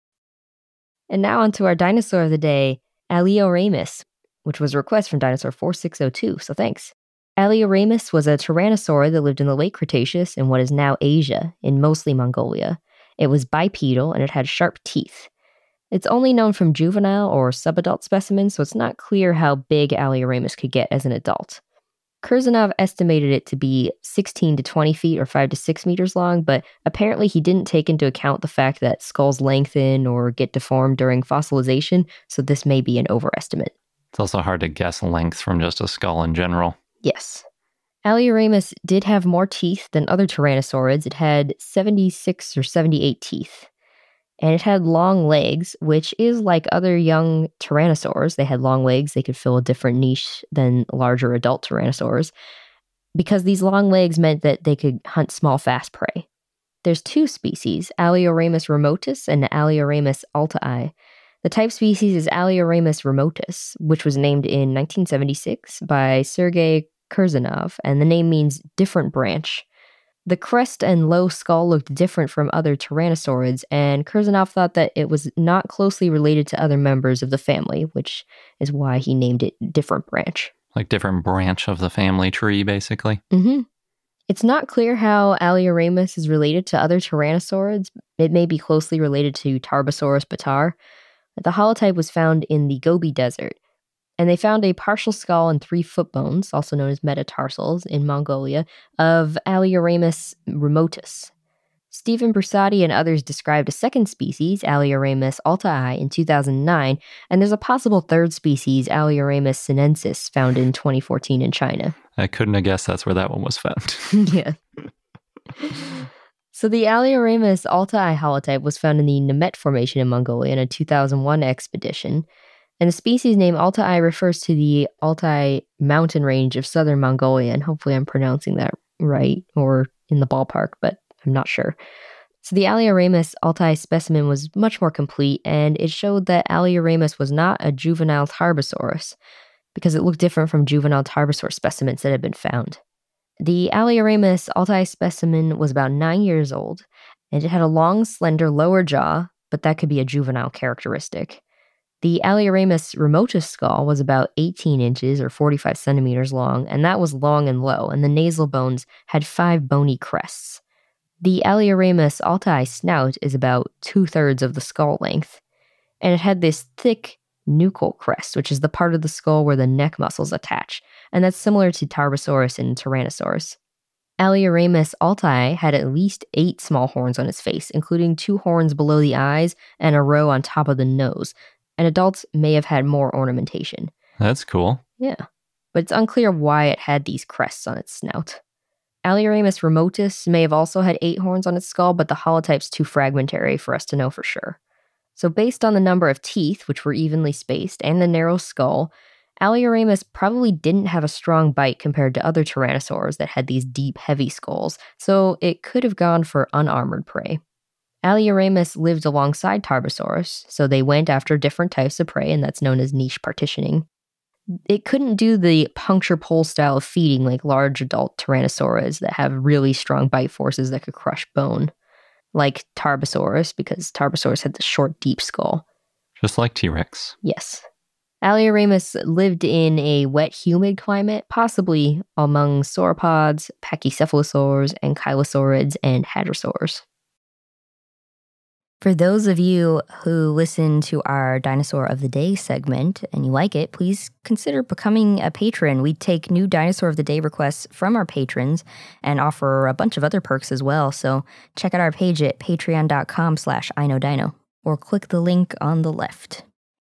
Alioramus: Dinosaur of the Day
For the full episode where this dinosaur of the day was originally featured check out

Tyrannosaurid that lived in the Late Cretaceous in what is now Asia (mostly Mongolia)
Bipedal, and had sharp teeth
Only known from juvenile or sub-adult specimens, so not clear how big it was as an adult
Kurzanov estimated it to be 16 to 20 ft (5 to 6 m) long, but apparently he didn't take into account the fact that skulls lengthen/get deformed during fossilization, so may be an overestimate
Had more teeth than other tyrannosaurids
Had 76 or 78 teeth, more than other tyrannosaurids
Had long legs, which is like other young tyrannosaurs (had long legs and could fill a different niche than larger, adult tyrannosaurs)
Long legs meant it could hunt small, fast prey
Two species: Alioramus remotus and Alioramus altai
Type species is Alioramus remotus
Named in 1976 by Sergei Kurzanov
Name means "different branch"
Crest and low skull looked different from other tyrannosaurids, and Kurzanov thought it was not closely related to other members of the family (hence the name "different branch")
Not clear how its related to other tyrannosaurids, may be closely related to Tarbosaurus bataar
Holotype was found in the Gobi Desert (Nogon-Tsav locality)